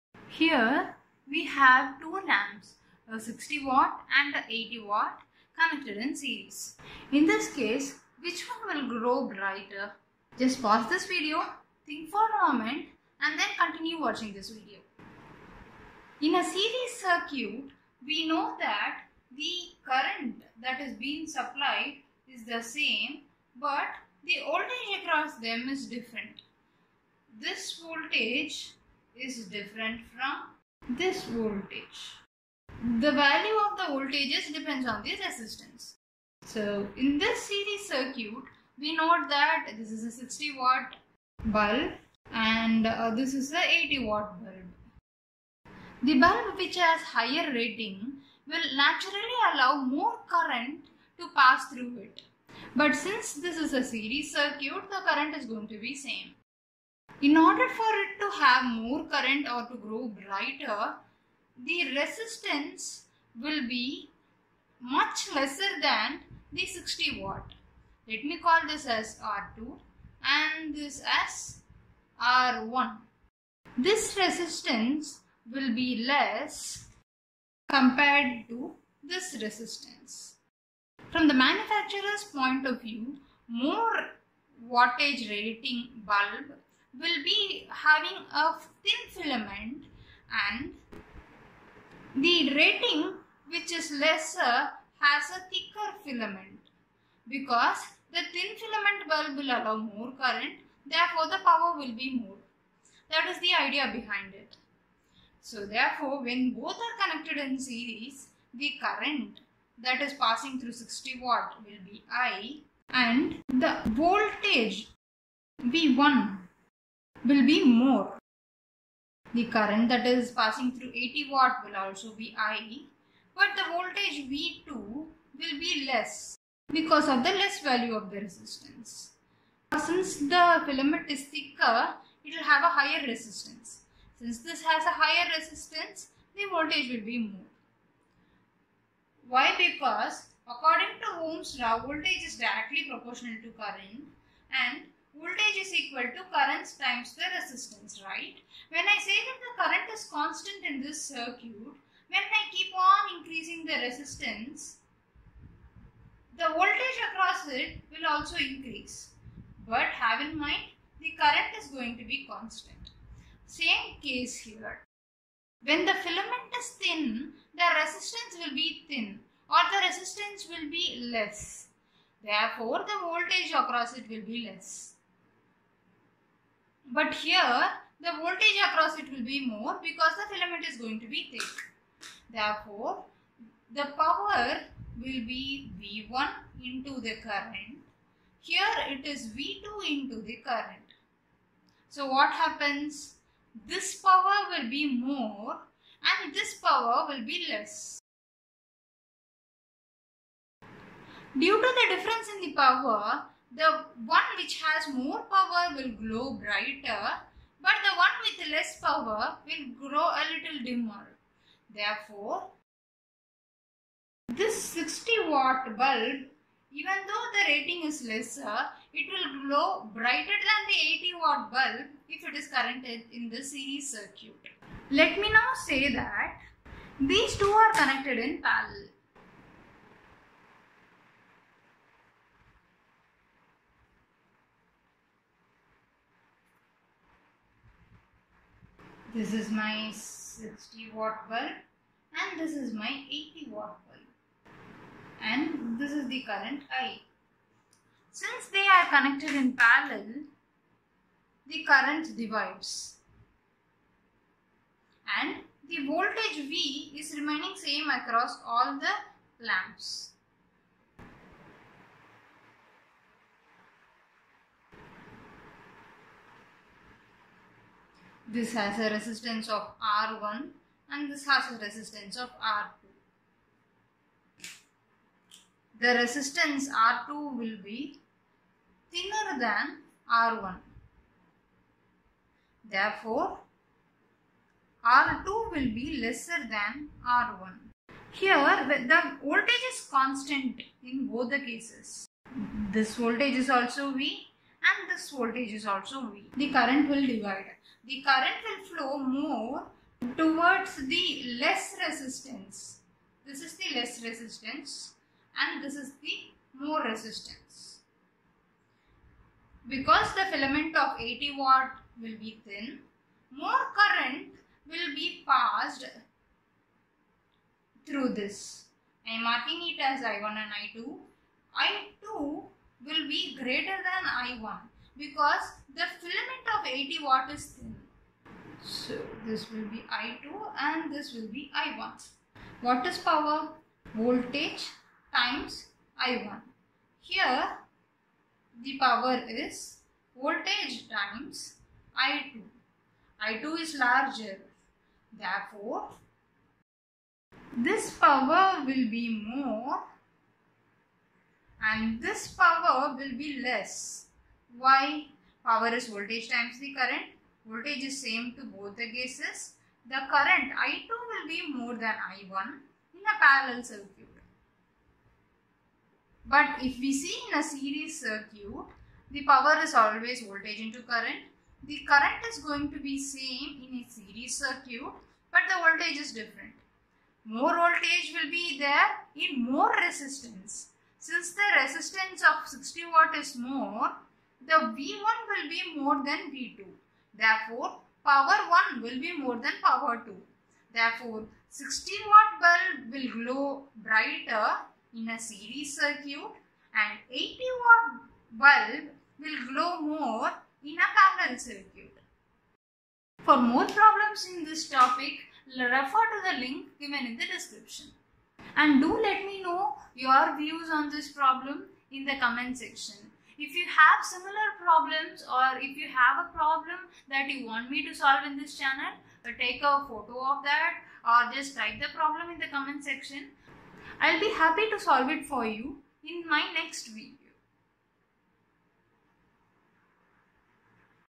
Two lamps 60 W and 80 W are connected in series. Which one glows brighter ?
0:00 : Which One will glow Brighter? - Explaination
1:52 : Current / Voltage relationship
3:13 : Answer is here!!! But, watch it from 1:52 for the explanation.
3:43 : What if 60W and 80W are in parallel?
5:50 : Summary